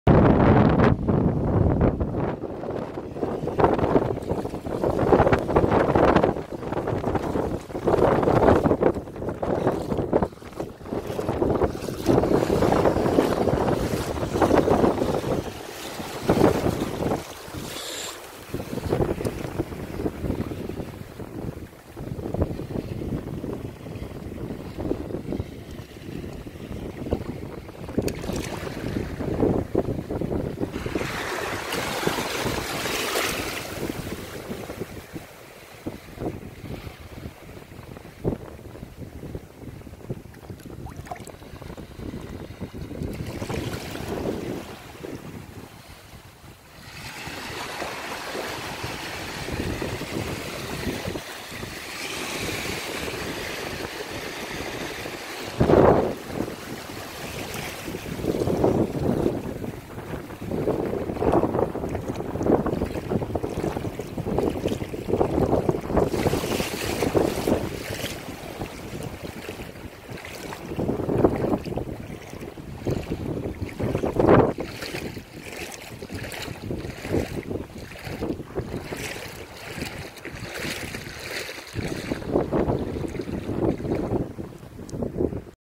33cm Javelin,Barred-Grunter Release Hervey Bay Qld
Caught On Deadbait Prawn Released Into Surf Point Vernon Qld